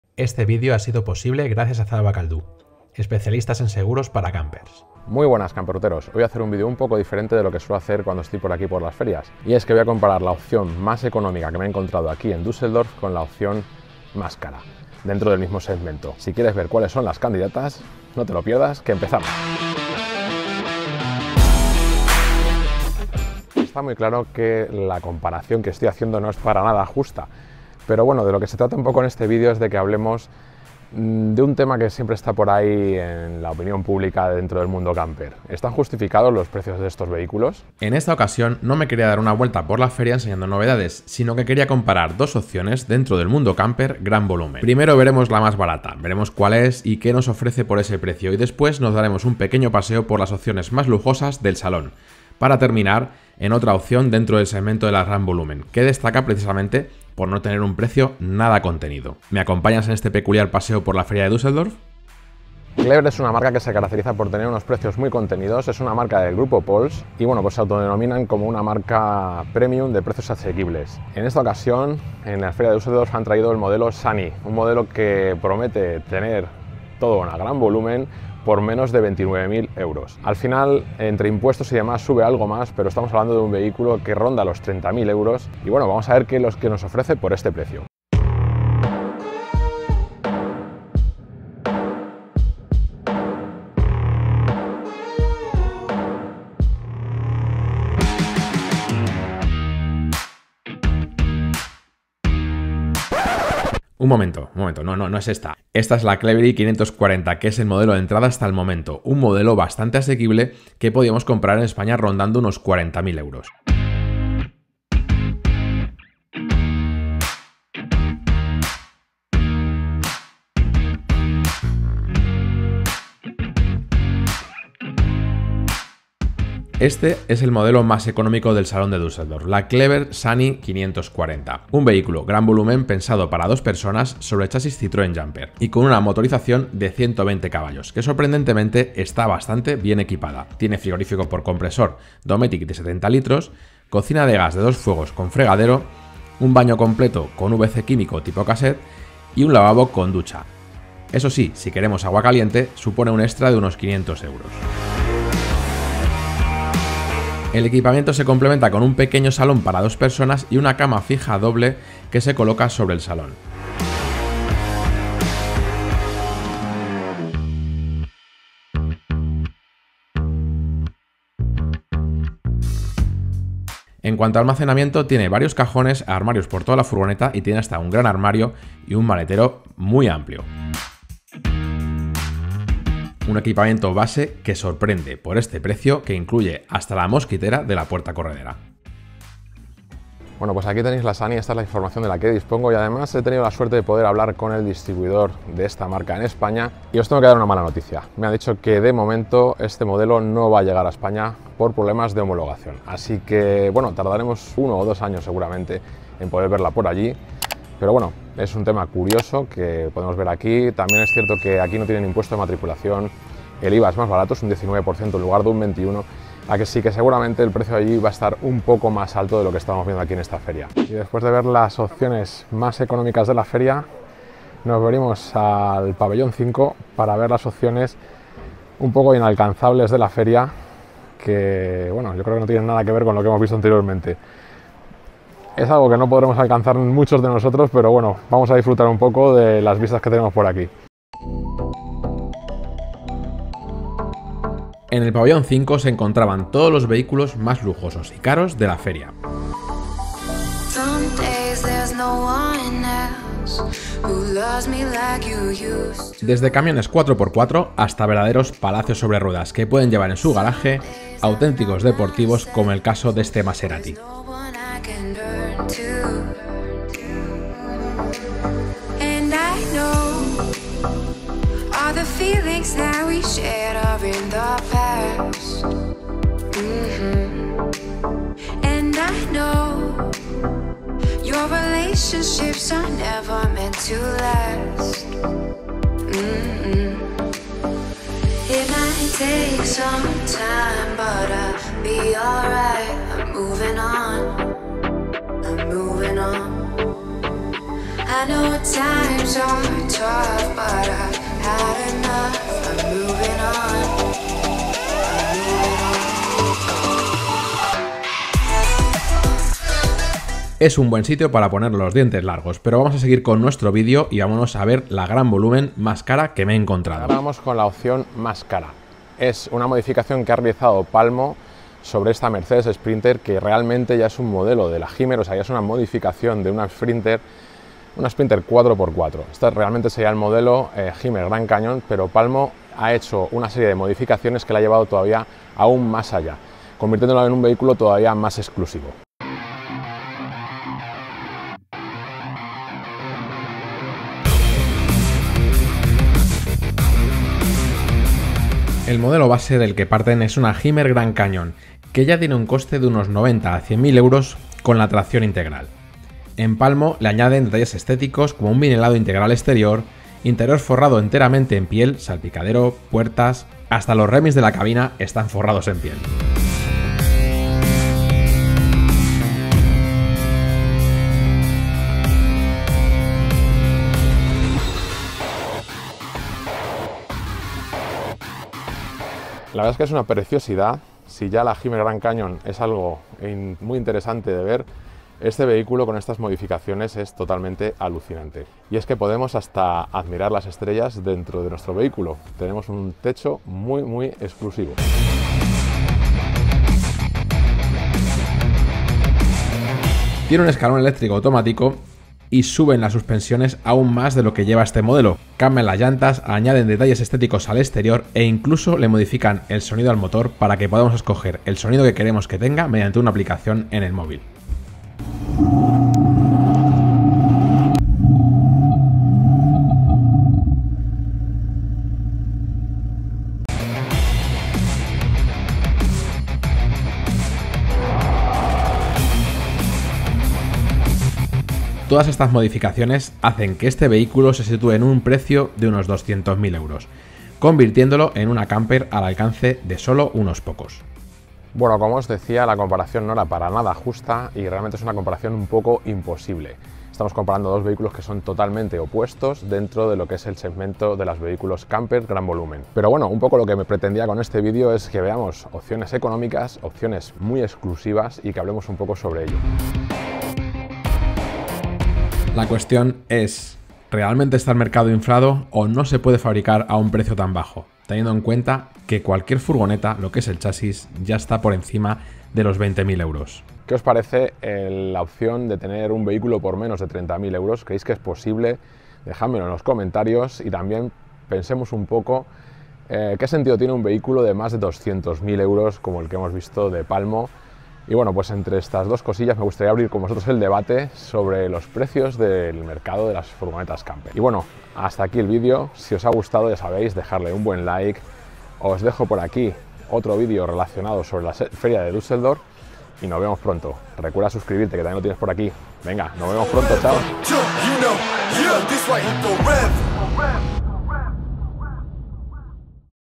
✅ Furgoneta CAMPER BARATA 😱 por menos de 30.000€ ❌ VS la opción MAS Cara de Düsseldorf 🚐 #236
Es posible comprar una CAMPER nueva por menos de 30.000€?

💚❤️💚👇Aquí TODA la INFO
Me doy una vuelta por el Salon de Düsseldorf buscando la opción más económica y la opción más cara ¿Cuales serán?

🕢 Aquí la estructura del Vídeo 🕢:
0:00 - 1️⃣ Intro
0:24 - 2️⃣ ¿Están caras las Furgonetas CAMPER?
1:03 - 3️⃣  La CAMPER más BARATA
3:40 - 4️⃣  Paseo por la zona lujosa del Salón
5:17 - 5️⃣  La CAMPER más CARA
7:22 - 6️⃣  Conclusiones

¿Quieres un seguro para tu furgoneta Camper?